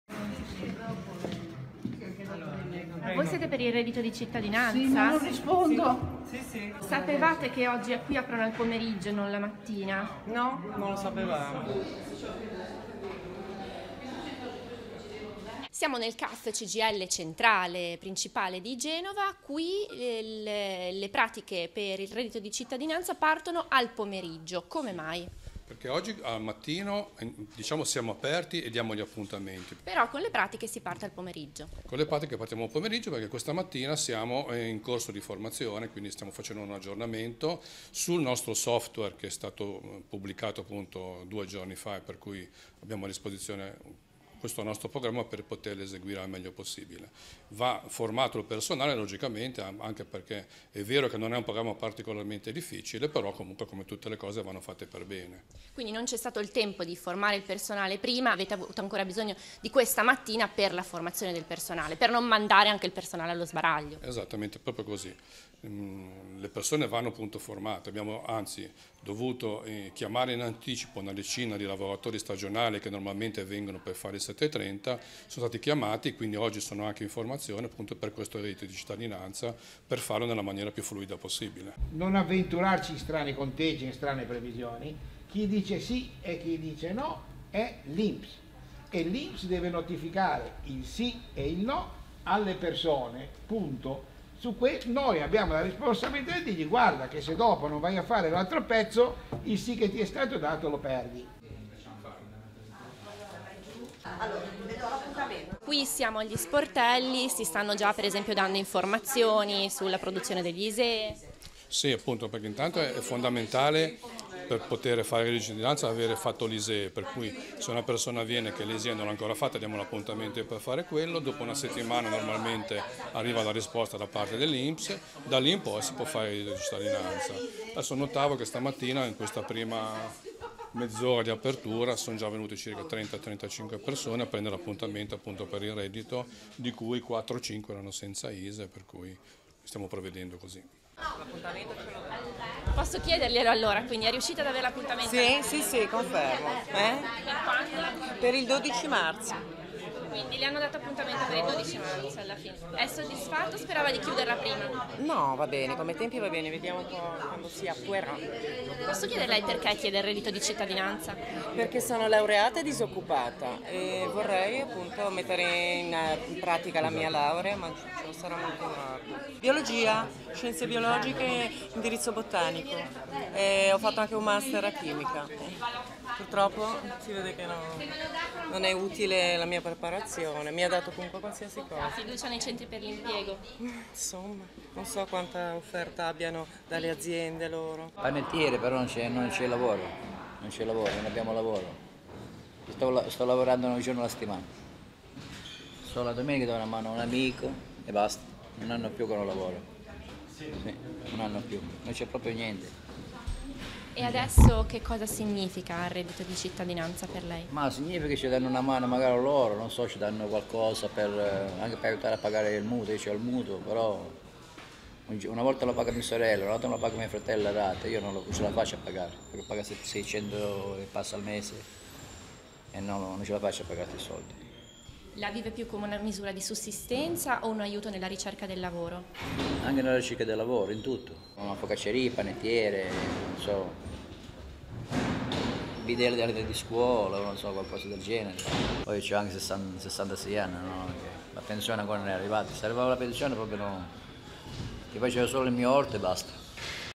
Reddito di cittadinanza, ''Poco tempo per formare il personale'':Caaf di Genova le domande slittano
Parte la corsa per la presentazione delle domande per il reddito di cittadinanza, "ma il personale degli uffici deve essere formato adeguatamente, ed è successo tutto così in fretta che non ne abbiamo avuto il tempo", spiega Luciano Demelas, responsabile del centro autorizzato di assistenza fiscale Cgil di Genova (Caaf), centri che insieme a Inps e Poste sono i punti di raccolta delle domande. Così qui l'apertura degli sportelli slitta al pomeriggio, e per tutta la mattina il personale, compresa una decina di lavoratori stagionali arrivati per l'occasione, è riunito per la formazione sul sistema di caricamento delle domande. Mentre le persone in coda - un trentina già all'apertura degli uffici - sono invitate a prendere un appuntamento per i giorni successivi. "Mi hanno detto di tornare il 12 marzo - racconta una signora - Va bene così, speriamo però che serva a qualcosa e si riesca ad ottenere un lavoro, io ho una laurea in Biologia ma sono disoccupata" .  .di Giulia Destefanis